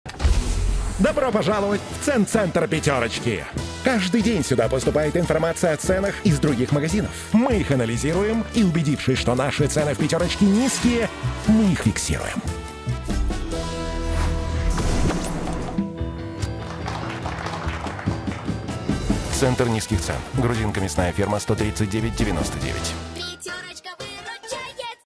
The Bytheway: «Пятёрочка» и «Родная Речь» рассказали о тайном подразделении торговой сети
Стало ясно, кто стоит за низкими ценами в магазинах сети «Пятёрочка»